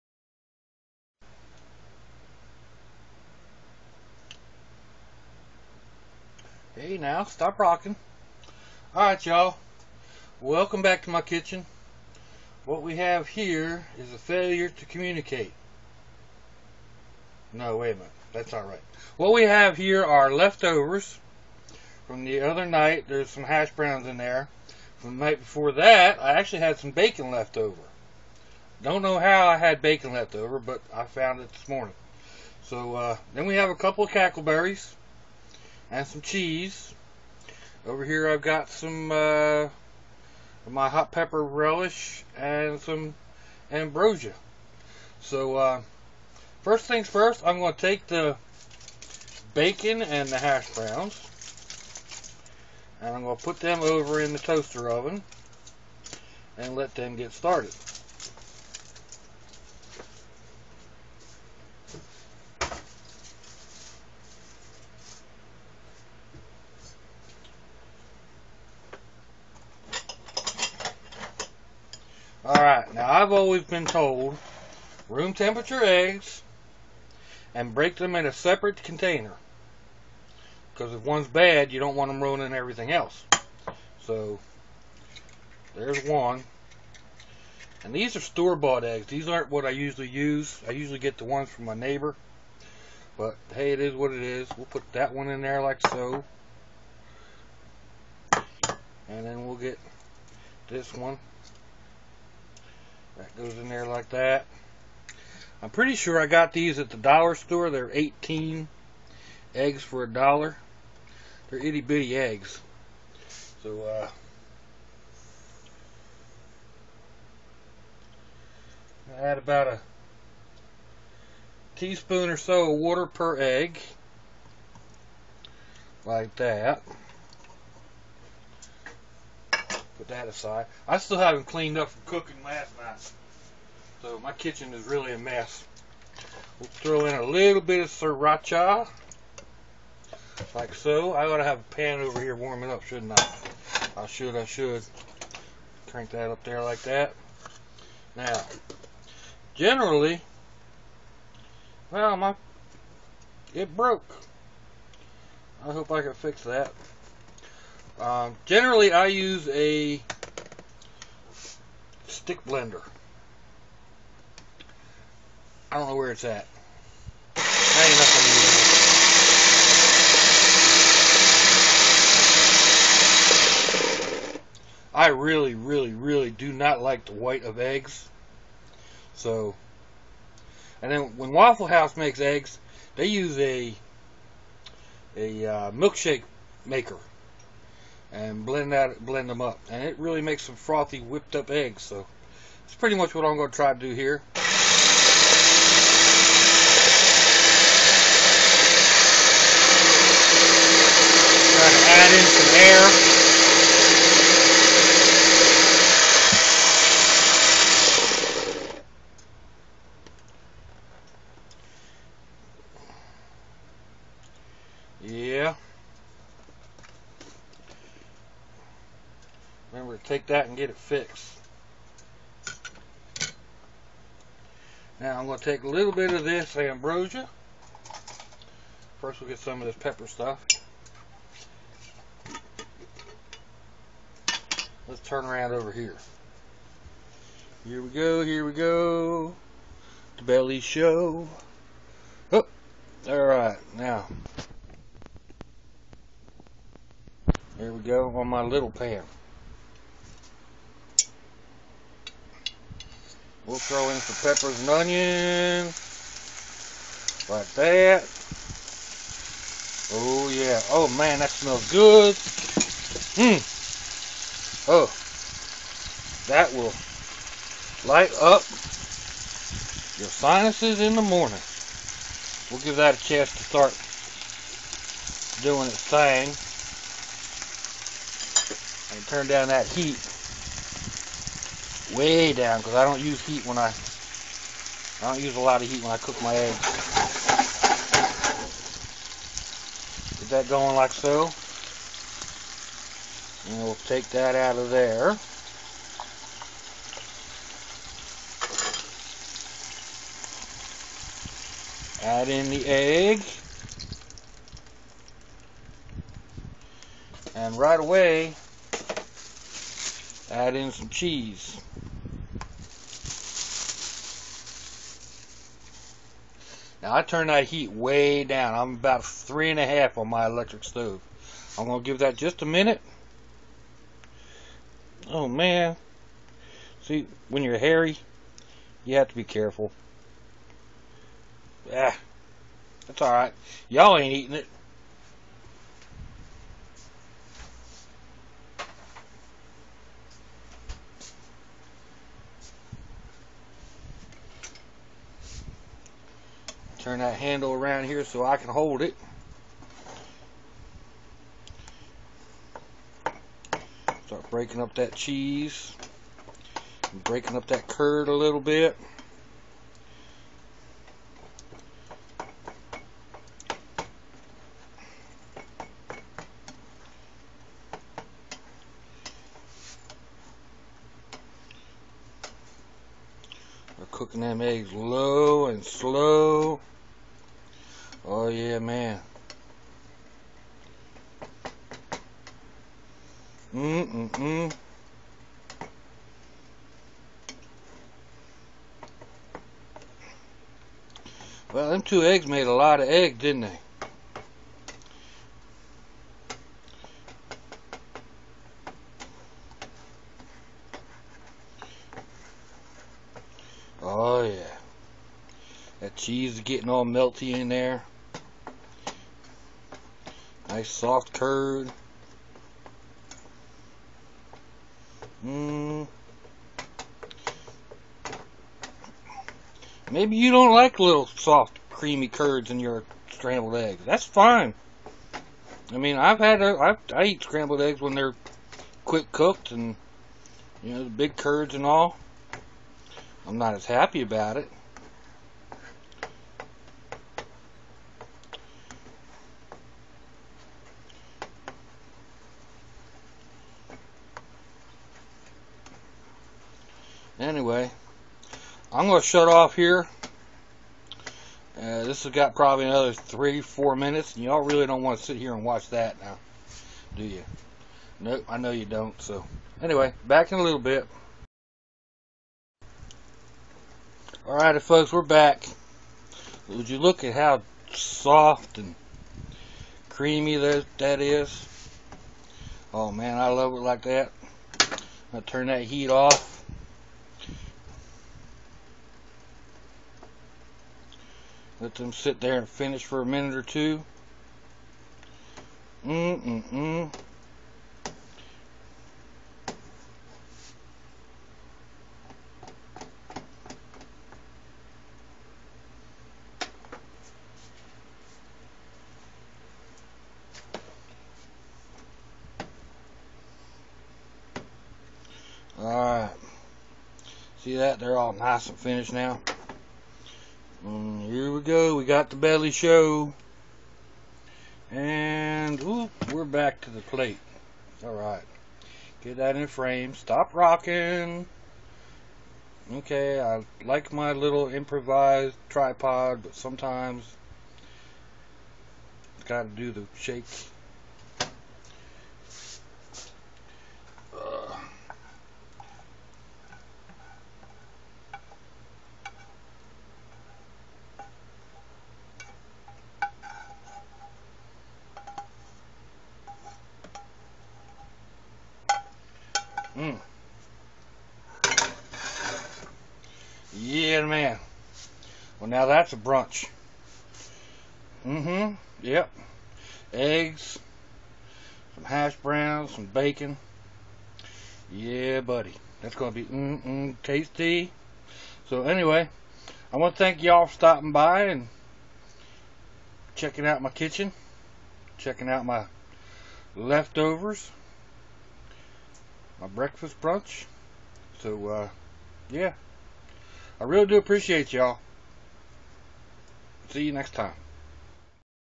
Cooking with Doc...super soft and creamy scrambled eggs with cheese
Whipped to an airy froth and cooked low and slow, my scrambled eggs  virtually melt on your tongue. I think the time is well spent